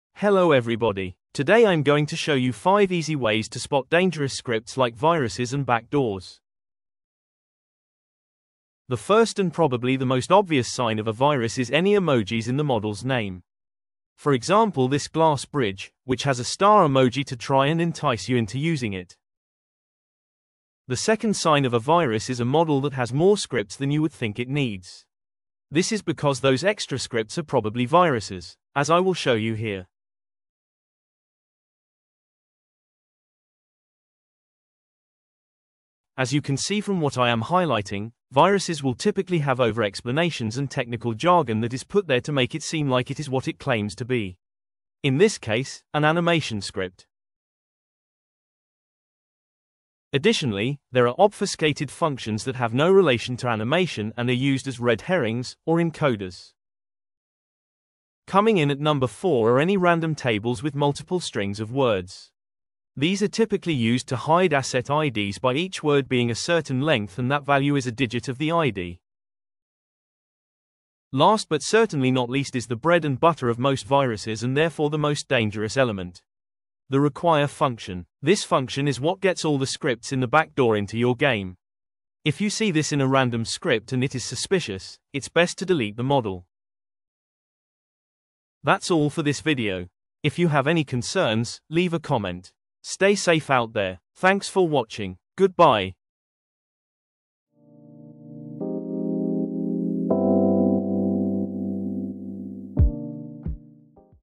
Identifying Viruses Made Simple in Roblox Studio (2025)
Today I Tell You How to EASILY Find Viruses in Free Models in Roblox Studio!